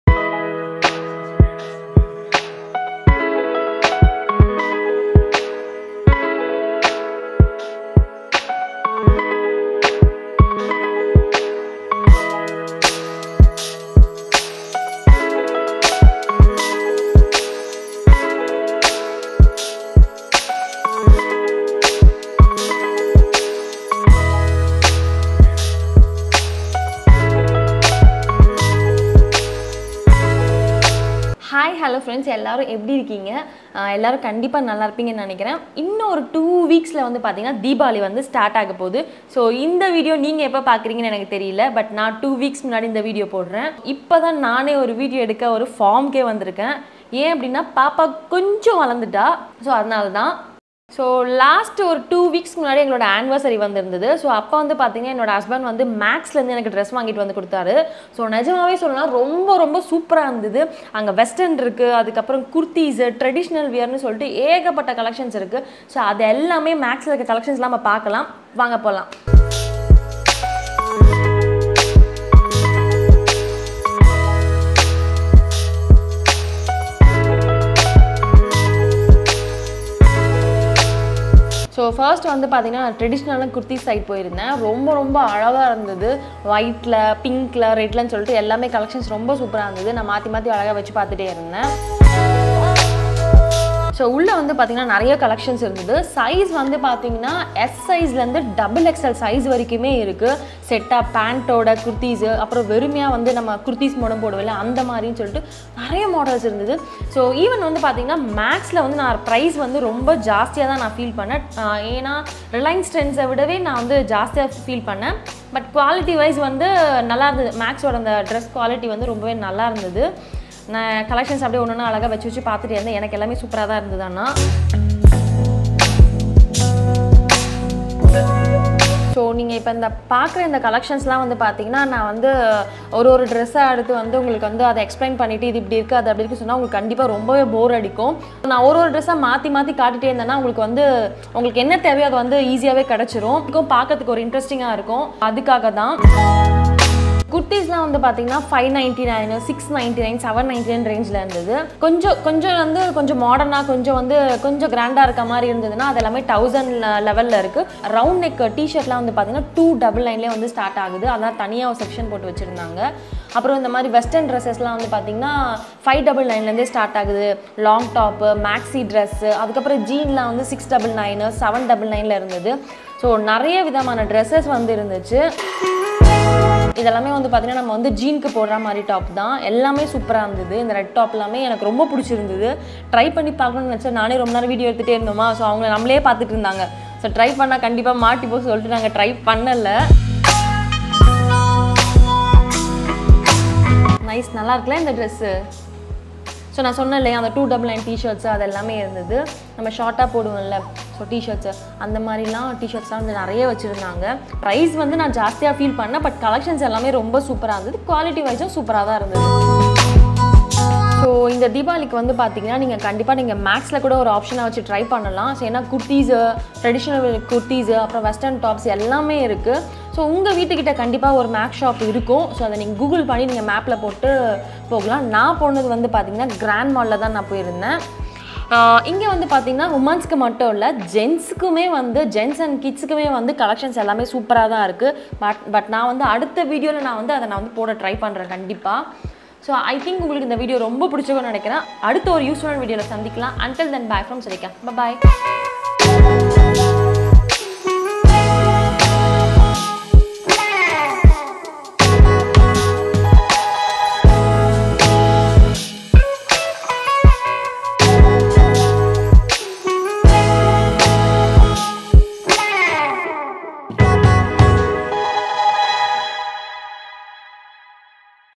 Best Office wearing Kurtis in chennai | Money for Quality!
In this Video I gonna show you the dress collections of Max Fashion in Chennai.

Collections are available for Men, Women and Kids as well.